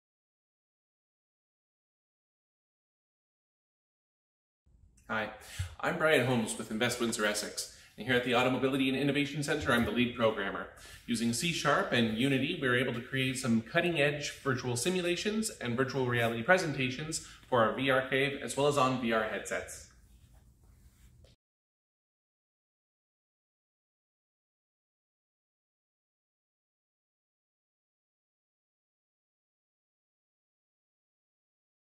Windsor-Essex RTDS - Meet Bryan Holmes, Software Developer, Simulation Team
Connect with Invest WindsorEssex team member Bryan Holmes, Software Developer, Simulation Team. Using C# and Unity, Invest WindsorEssex is able to create some cutting edge vitual simulations and virtual reality presentations for our VR CAVE and VR headsets.